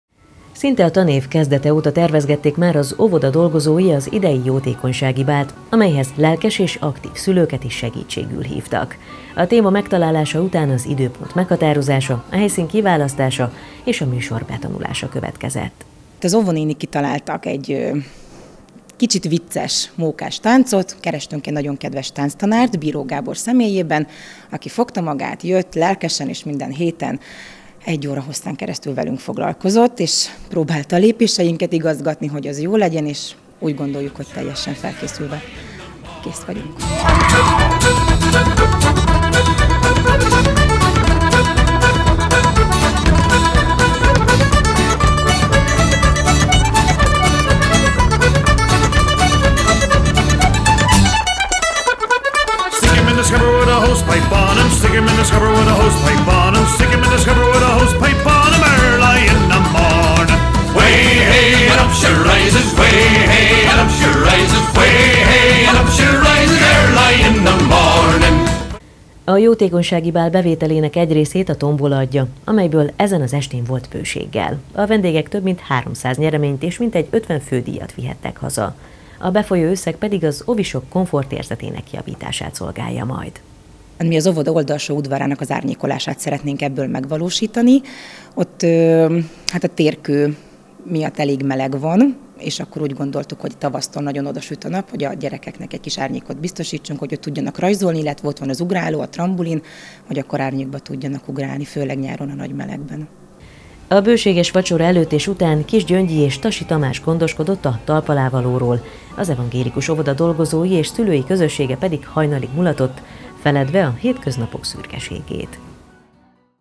Fedélzetre fel! - KEVI Harangvirág óvodájának bálja
Fedélzetre fel! Adta ki a hívószót a KEVI Harangvirág óvodája, és ezzel november 17-én szombaton el is indították a báli szezont Kiskőrösön. A tengerész bál résztvevői közel 180-an, megtöltötték a Fortuna éttermet.